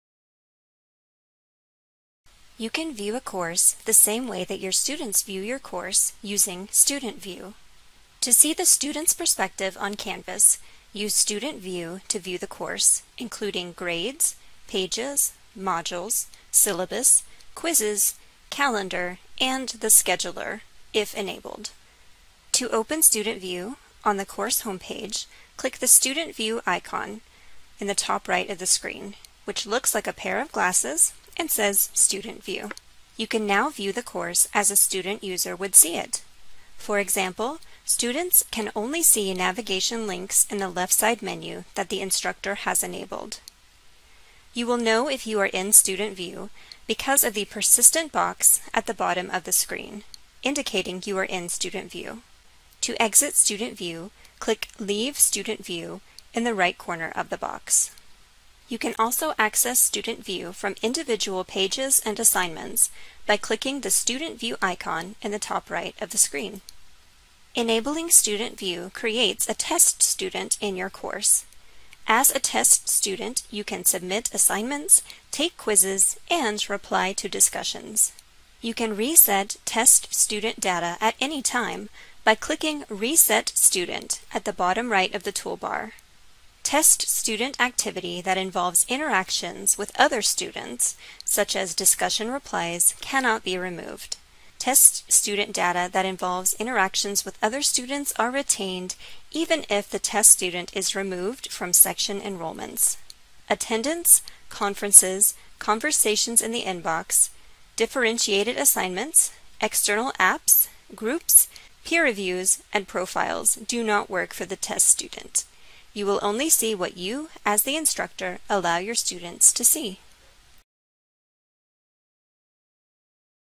Canvas Quick Tips: Use Student View In Canvas
You can view a course the same way that your students view your course through Student View. Student View is now available as a feature on assignments, pages, discussions, quizzes, etc.